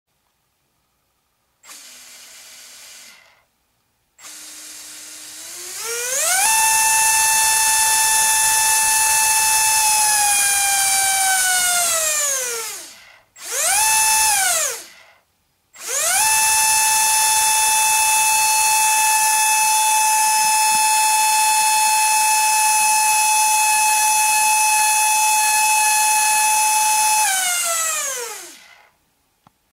JAS 39 Gripen SAAF EDF30 motor revving
2mm Depron Gripen with 30mm GWS ducted fan. Motor is Orion 1/36 scale 9500kv brushless motor. Test run with 2cells 400mah 20C.